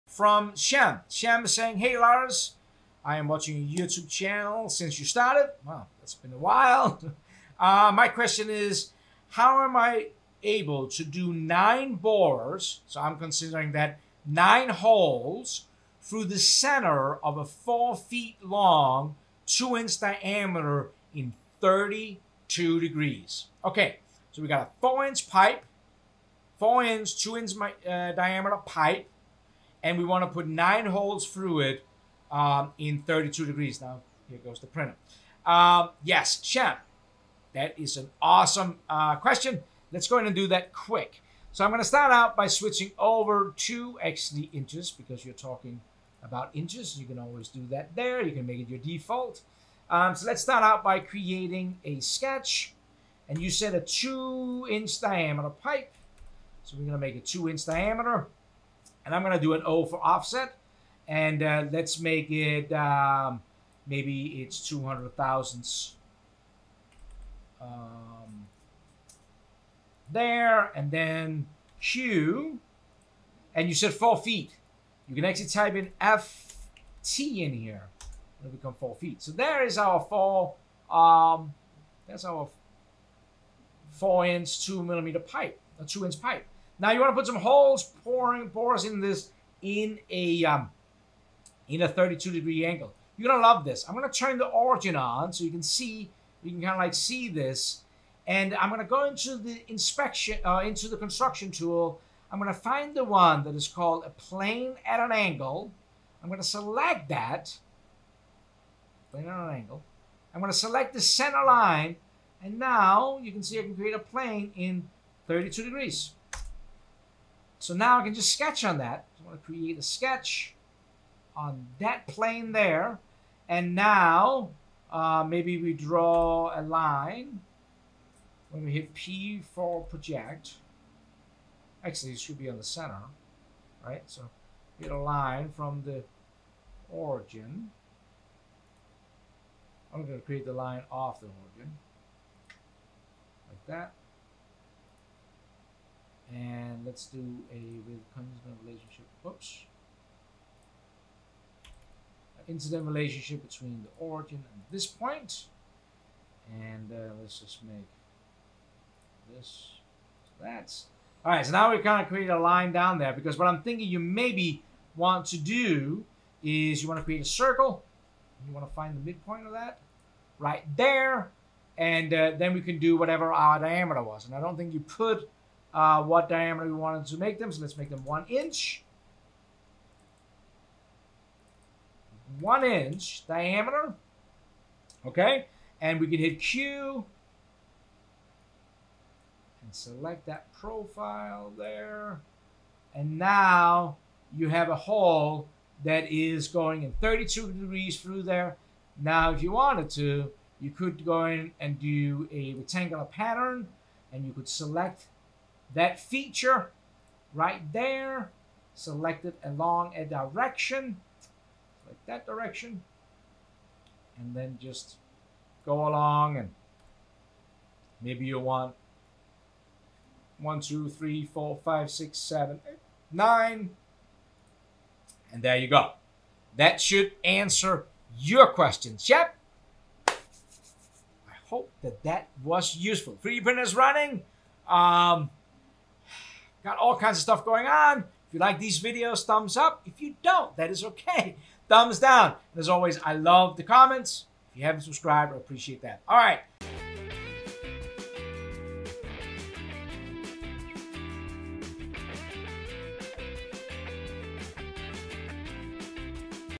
Fusion 360 — Model Holes in 32 degrees in a Pipe — Ask LarsLive
Model Holes in 32 degrees in a Pipe is a snippet of Sunday's Ask LarsLive.